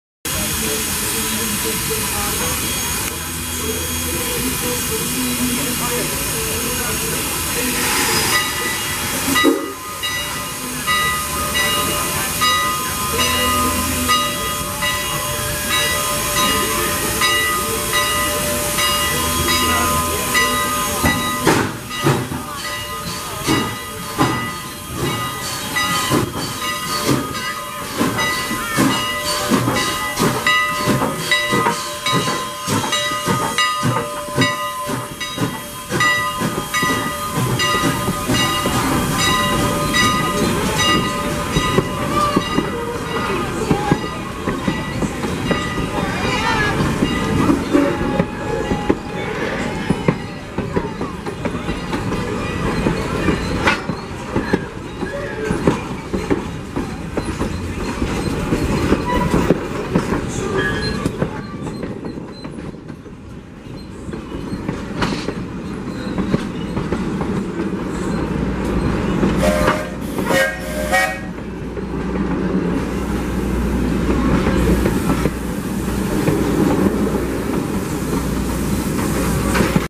Day Out With Thomas At Strasburg Rail Road 2024 Part 118
Mario/89 Departs East Strasburg Station.